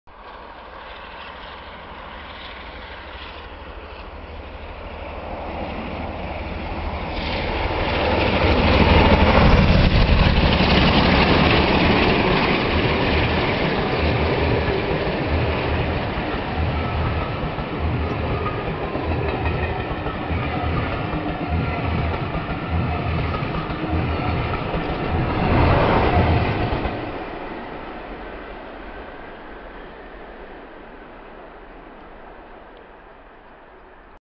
47804 & 47826 pass through Manors in the snow
West Coast class 47s 47804 & 47826 pass through Manors in Newcastle in the snow